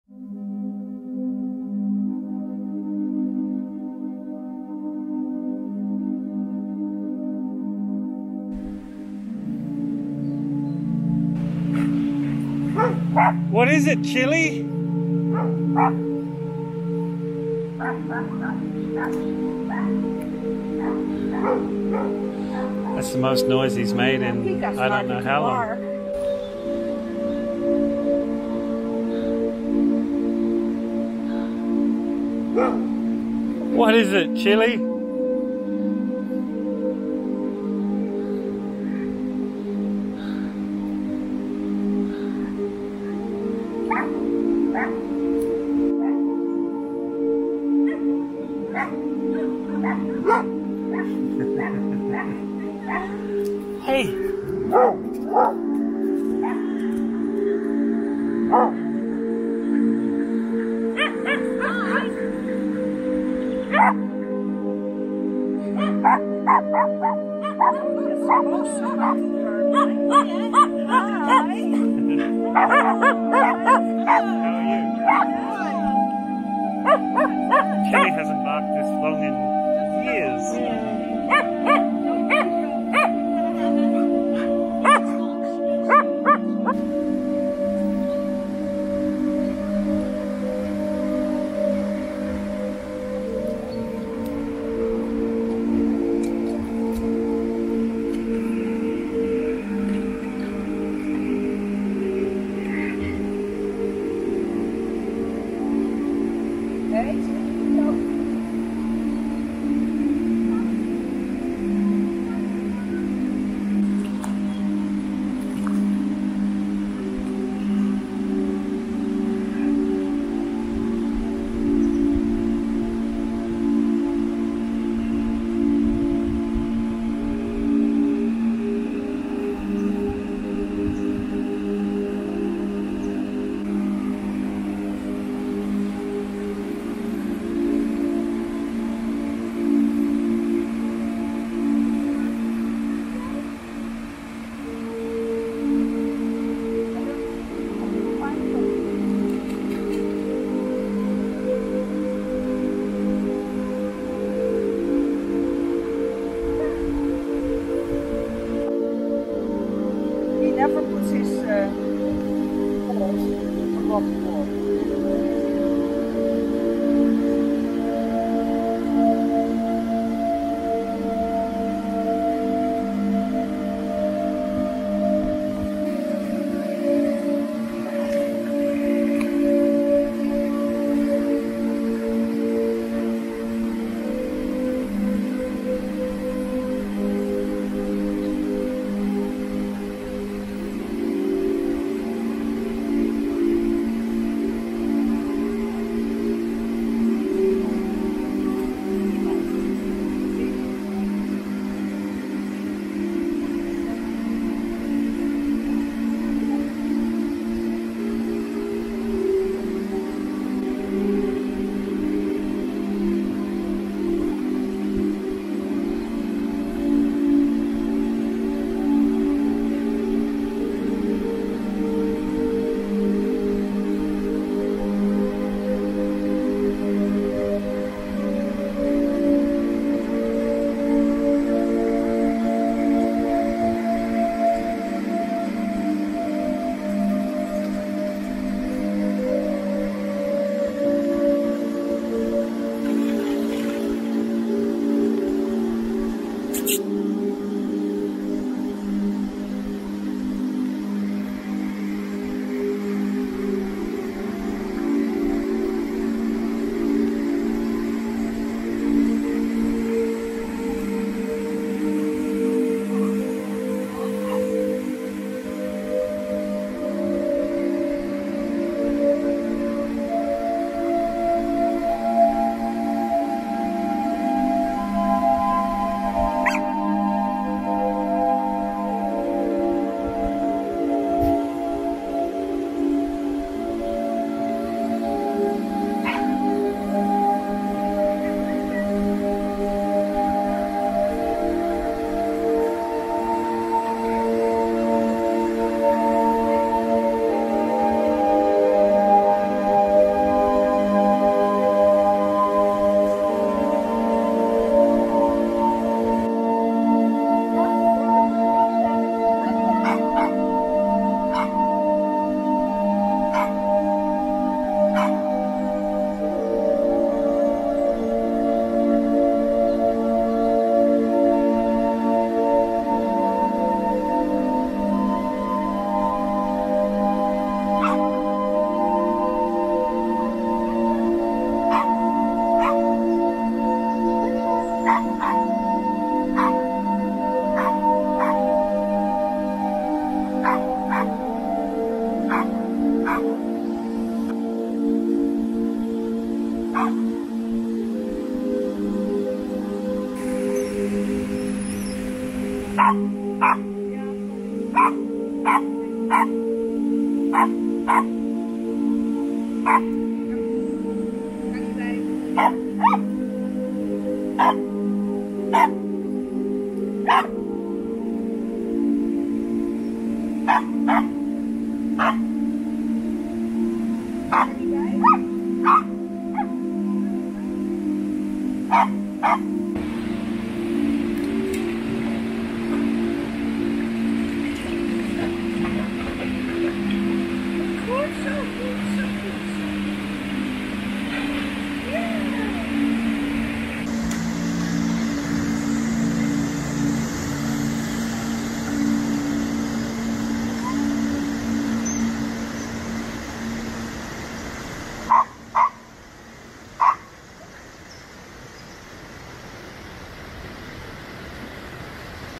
Chilly in a river
Chilly turned 14 this month and having travelled with me through 4 countries, living on a sail boat for 5 years and almost never being left home alone he is one of the calmest and gentlest animals I have ever known. He doesn't usually bark which gave me a surprise when he barked at the other dogs when they joined us for a puppy play date at the river.
Summer is here in Portugal and my host at Villa-amalia.weebly.com has been taking us to the river almost daily to let the dogs swim run and play, the temperature is in the low 30's and I know for sure Chilly loves just walking around in the water and cooling off.

Central Portugal is an amazing place and with spots like this to cool off its a great place to visit.